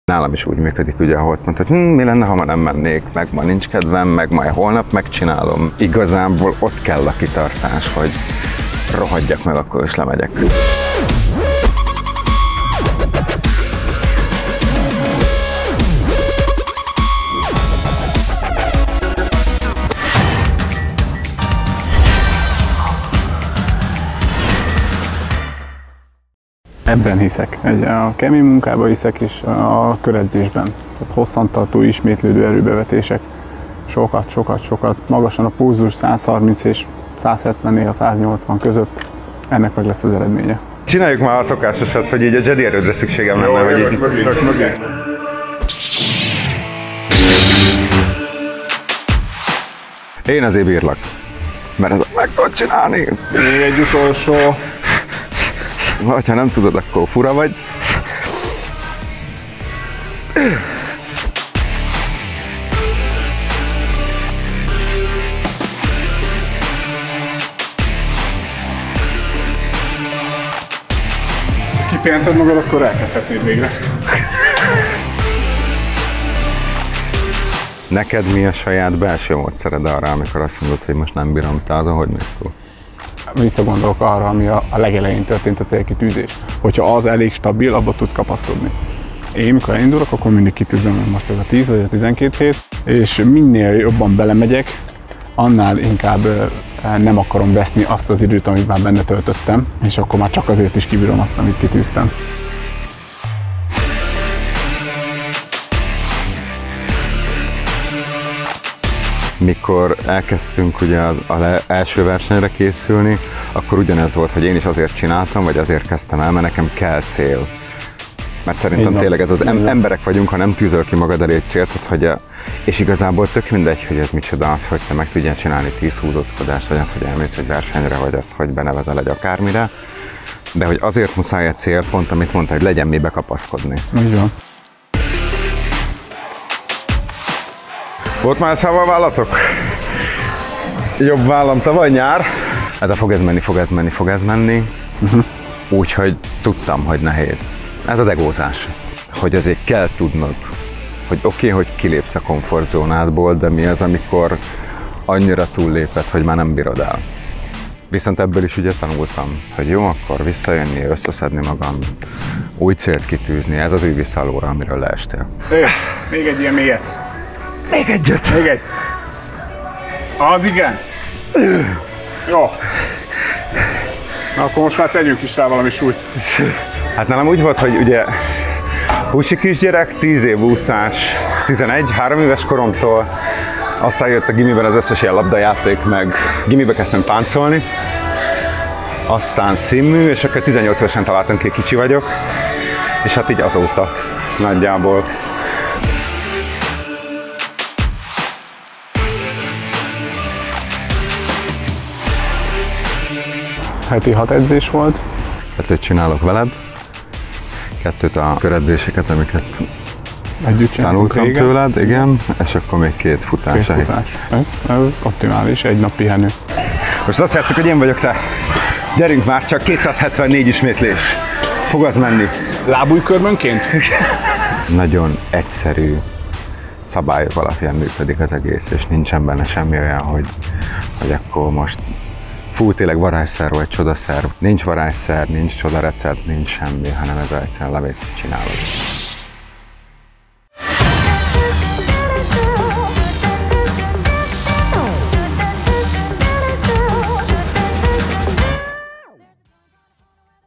BioTechUSA - Man maker: Eddz keményen! - 2. rész
A Man maker egy olyan crossfit gyakorlat, ami a test minden nagyobb izomcsoportját megmozgatja.
Videósorozatunk erről a nehezített négyütemű fekvőtámaszról kapta a nevét, ahol Magosi Zsomborral és Csórics Balázzsal, mint edzővel és tanítványával járjuk körbe azt a témát, hogy mennyi mindent tanulhatunk saját magunkról, a határainkról és a testünkről edzés során.

A második részben ellátogatunk a srácokkal egy konditerembe, ahol szemtanúi lehettek milyen egy edzés Zsomborral, Balázs pedig a testmozgással kapcsolatos viszonyáról mesél nektek.